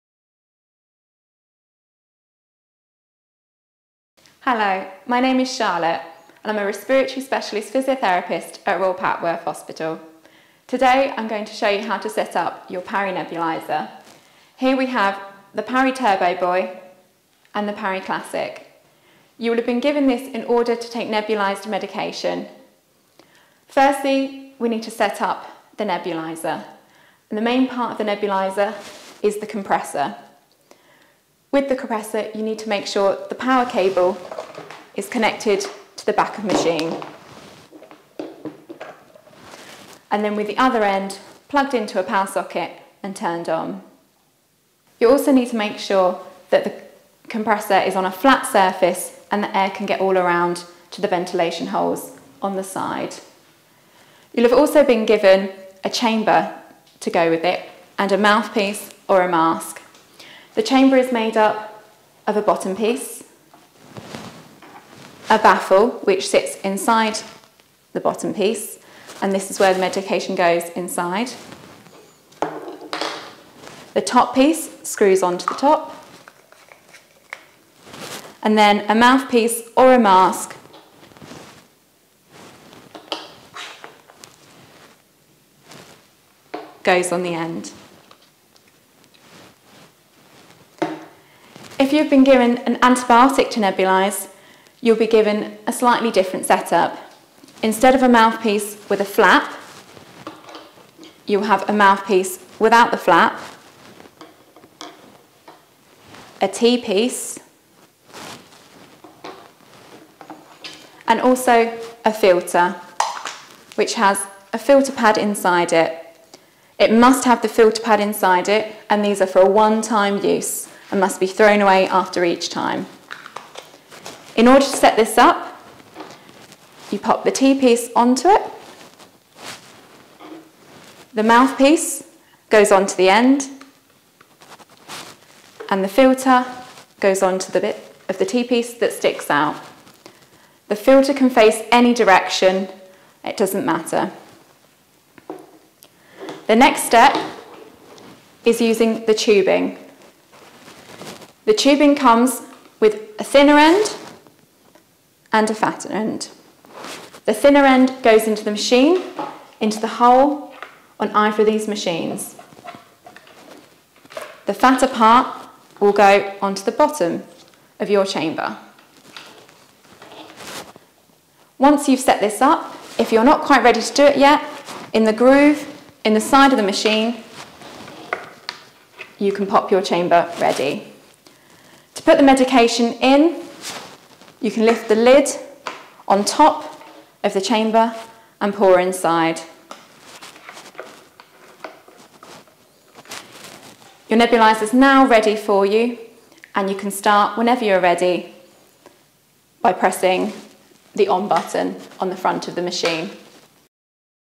Setting up your nebuliser
Respiratory physiotherapist Charlotte demonstrates how to set up a nebuliser.